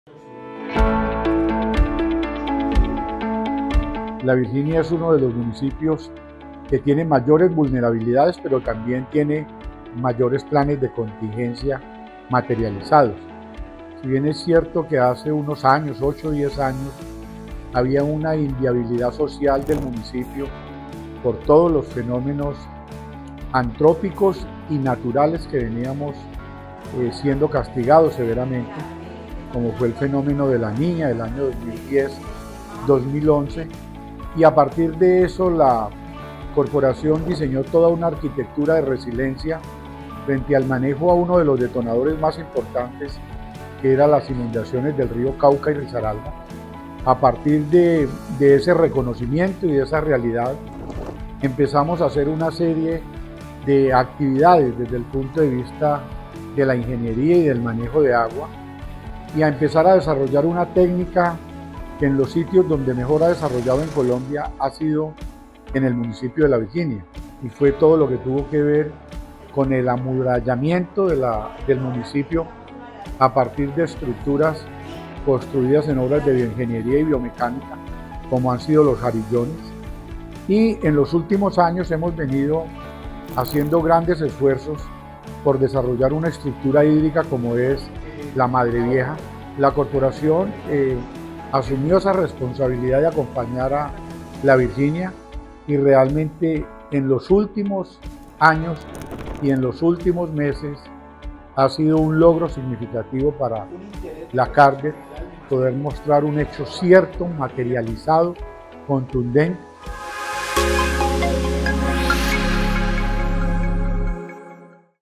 Mesa Ambiental La Virginia
🌳 "Cada árbol que protegemos en La Virginia es un regalo para todo el planeta", fue uno de los mensajes entregados por Julio César Gómez, director de la CARDER, durante la mesa ambiental. 🌍